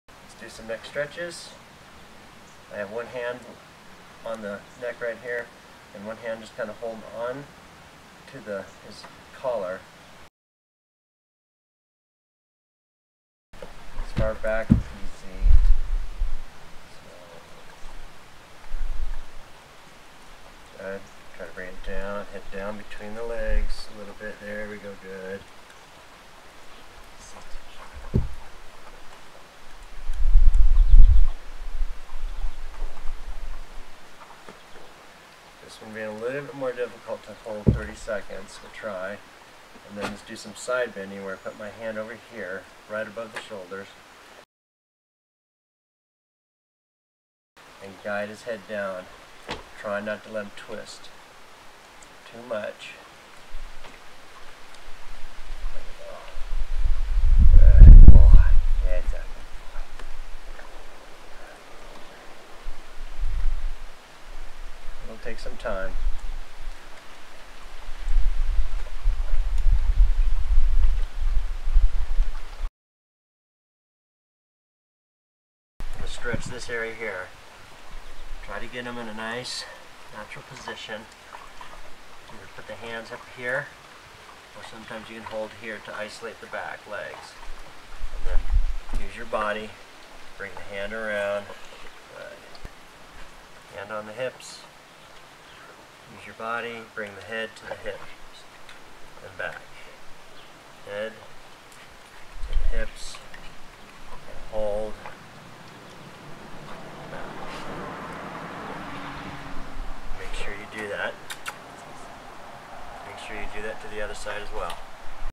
Canine Stretching for Neck and Spine
This is video #4 in our Stretching Safely series. Follow along as DogTread Canine Fitness Advisor/Coach provides techniques and tips for stretching your dog's neck and spine safely.